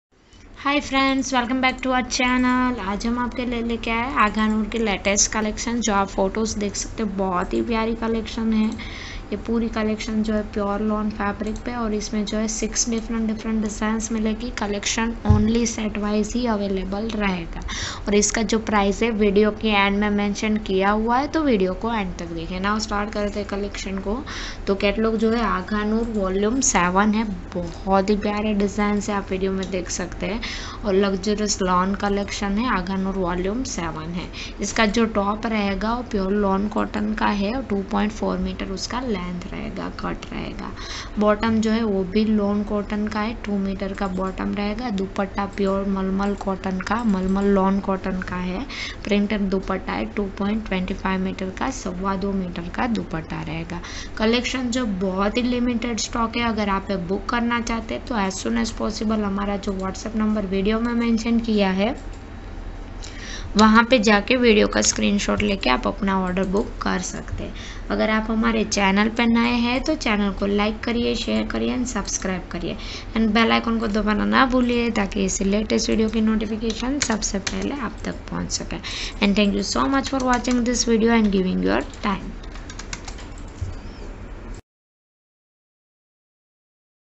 Agha noor Vol 7 Luxury Pure Lawn Dress materials || Latest Design Of Pakistani Lawn Suit 2022
Agha Noor Vol 7 Luxury Lawn Cotton Suit Collection

Catalog: Agha Noor Vol 7

Fabric Details: Top- Lawn Cotton (2.40 mtr)

Bottom- Lawn Cotton (2 mtr)

Dupatta- Pure mal mal Lawn  Printed (2.25Mtr)

Price: 430+GST